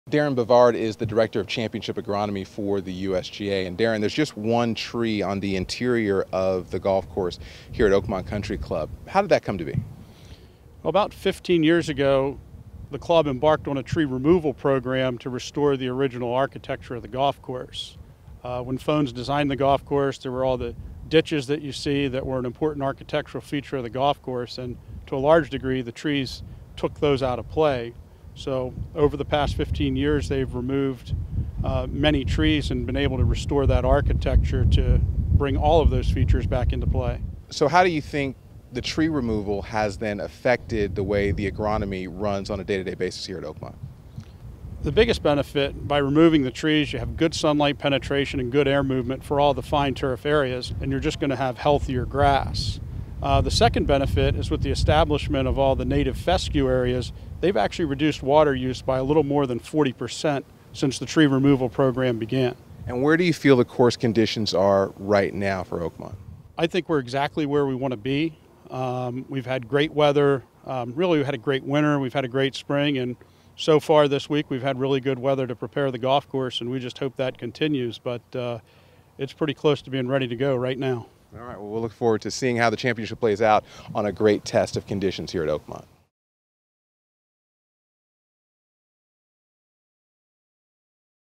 Oakmont: Ready for the World's Best
Favorable weather and tireless preparation have Oakmont primed for a memorable U.S. Open. For regular updates from the championship, visit usopen.com.